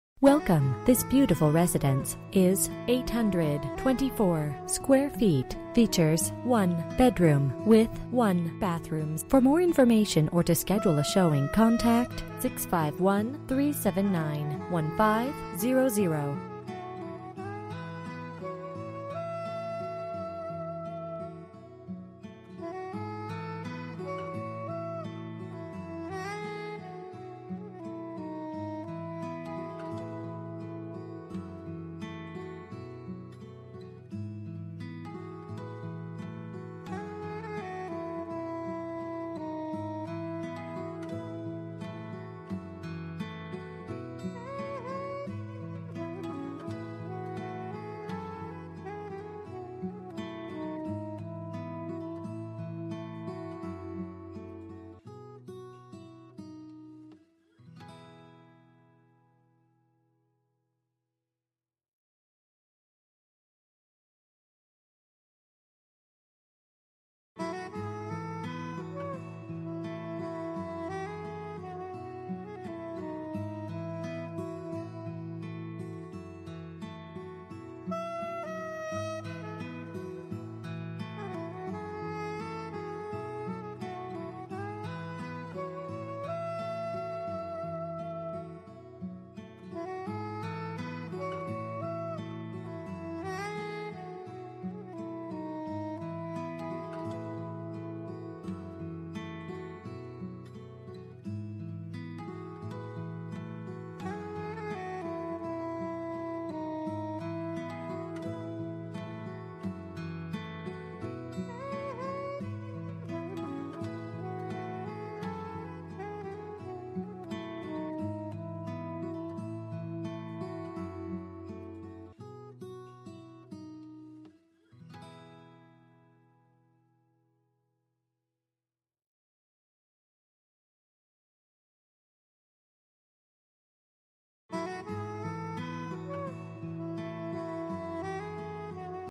Homes for sale - 10451 Greenbrier Road # 113, Minnetonka, MN 55305
Move-in ready 1 bedroom, 1 bathroom condo at The Ponds! First floor unit for easy access inside the secured building yet a safe distance up from the ground to the deck on the back.Tasteful kitchen features SS appliances, upgraded countertops & butcher-block breakfast bar plus pantry off the entry. Living/dining room combo including coat closet & spacious deck overlooking Minnehaha Creek and grounds. Spacious bedroom with walk-in closet, recently updated bathroom & side by side washer/dryer with storage in closet. Enjoy the great community amenities including underground heated parking with car wash, outdoor pool, tennis courts, party room, & personal storage locker. Updated electrical as well. Association dues cover heat, water/sewer & cable/internet too! Great location close to trail, parks, shopping, bus line & more!

Bedrooms: 1
Bathrooms: 1
Square Feet: 824
Price: $130,000

For more information about this property, please contact Sandy Erickson at or . You can also text 6184641 to 67299.